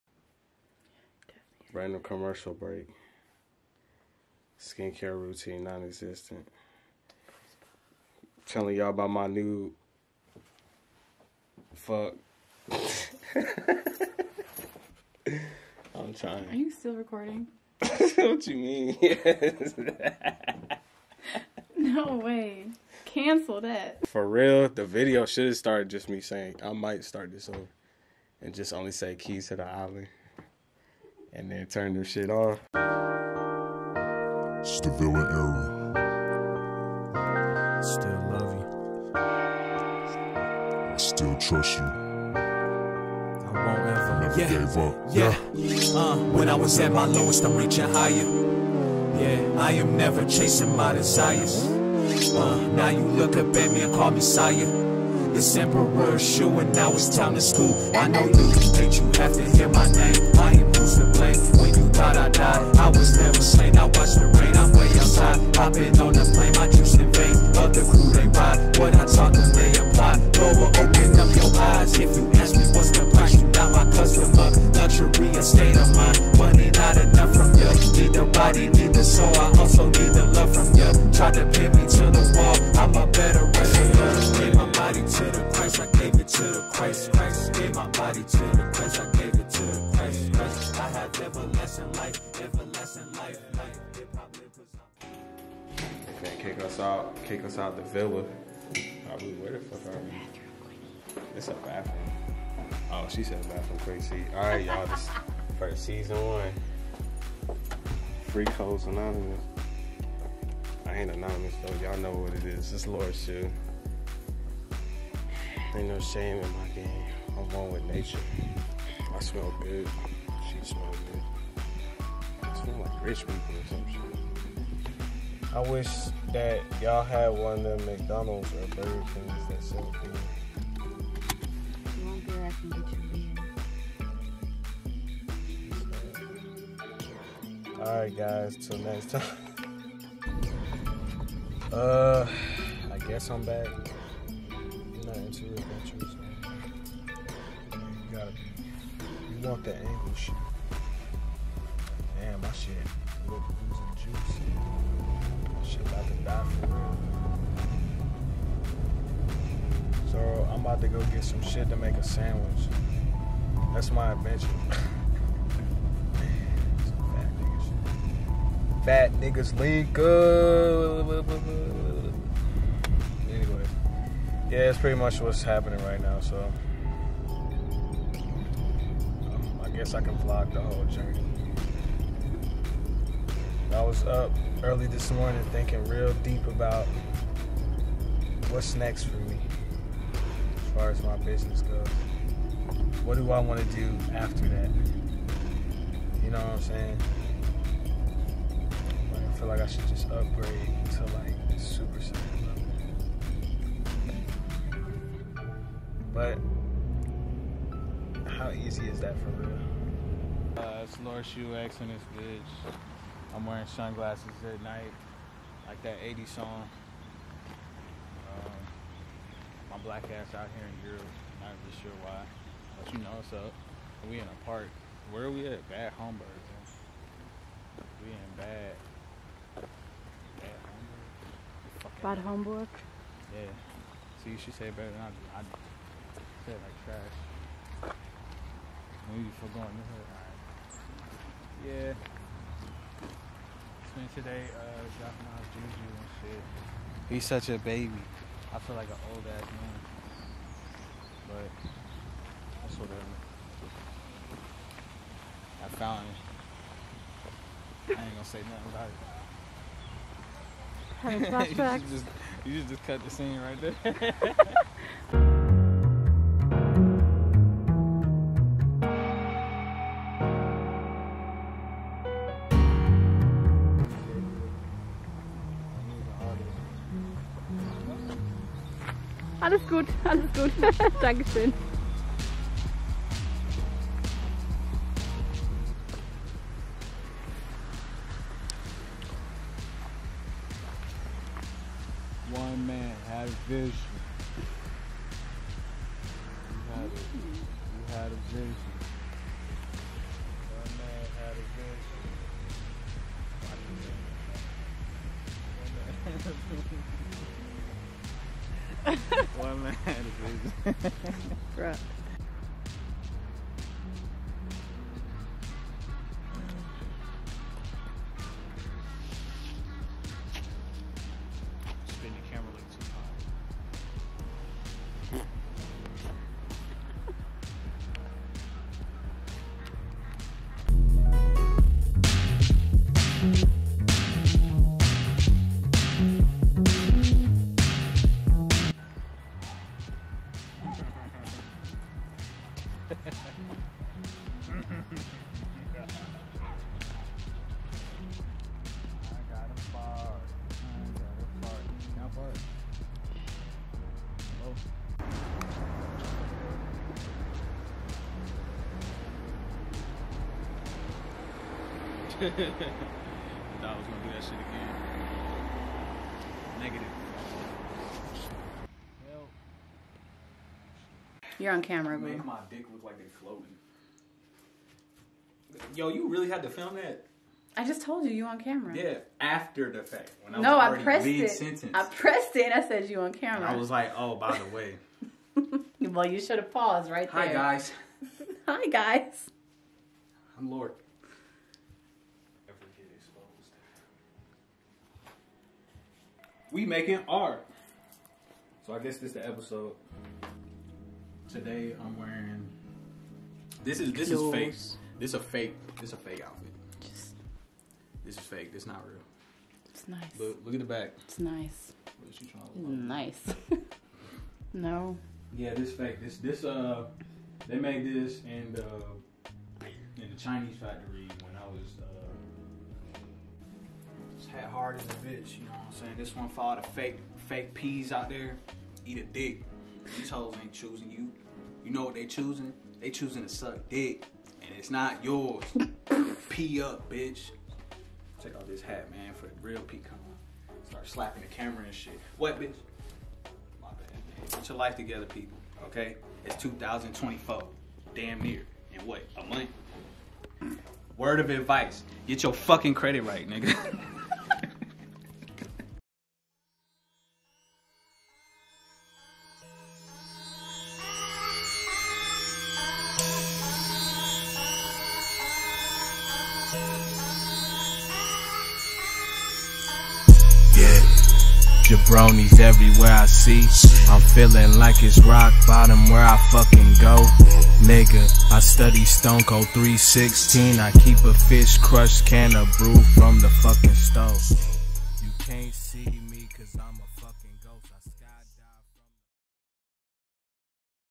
Season 2 Episode 5: Keys to The Island
Hybrid Manual is a magazine written by Lord Shu to offer value to people of all ages through knowledge. The topics in the magazine cover a general view of life and include examples of moments in Lord Shu’s life where he applied the written material known as, “Keys to The Island”.

All music in the video is produced by Lord Shu & ShuLovesRebels